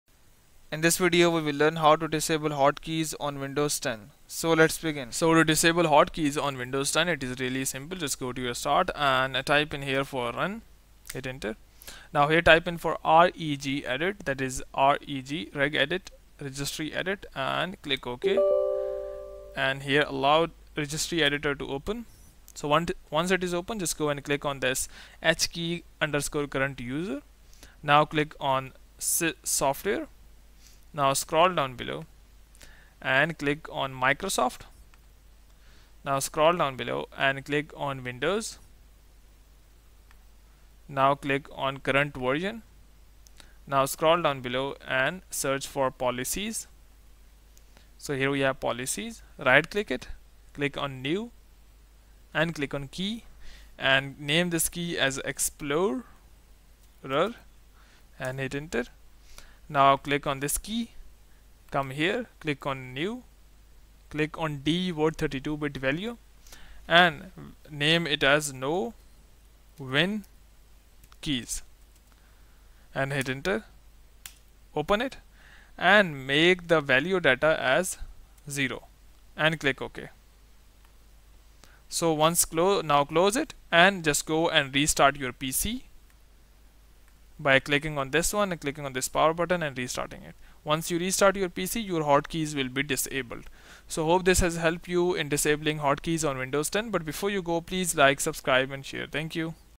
How to Disable HotKeys Windows 10 Dell Lenovo HP (Quick & Easy)
Learn How to Disable HotKeys Windows 10 UPDATED Dell, Lenovo, HP. It is simple to turn off hotkeys on Windows 10, follow this video.

0:00 Intro
0:05 Disable HotKeys Windows 10